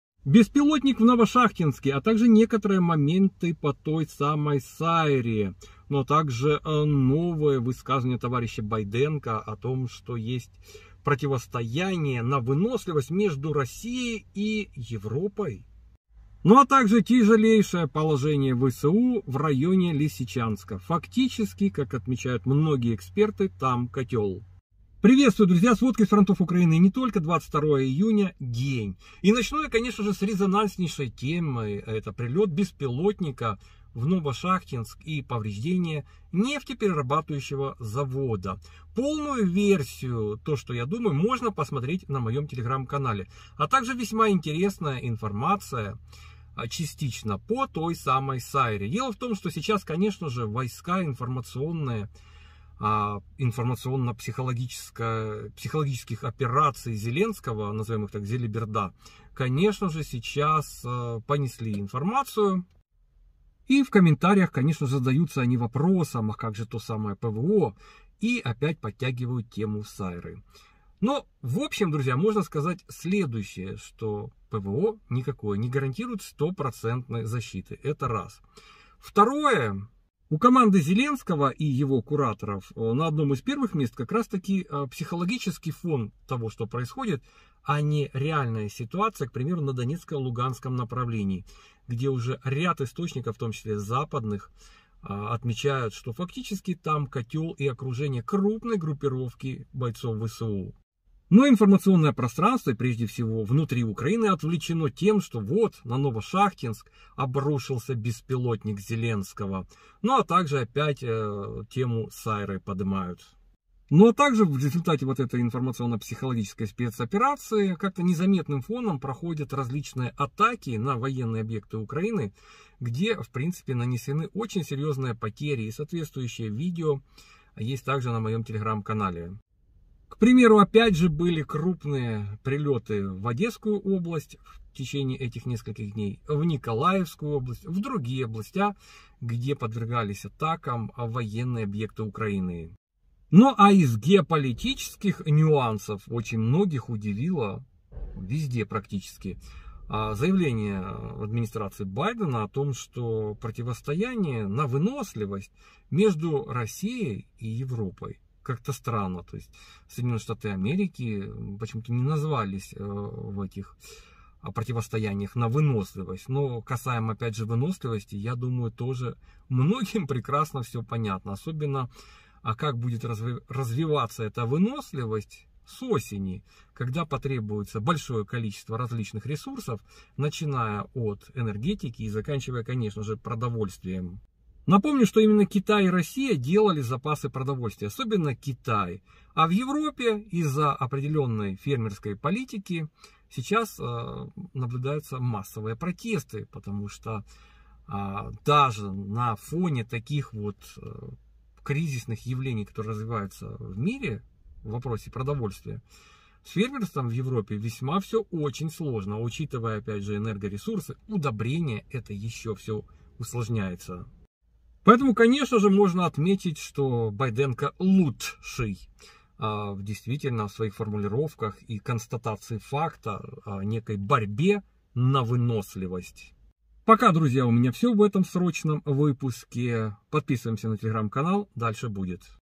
Тамир Шейх — БИTBA за Украину ¦ 22 июня
• БИTBA за Украину! 22 июня БЕСПИЛОТНИК атаковал РОСТОВСКУЮ область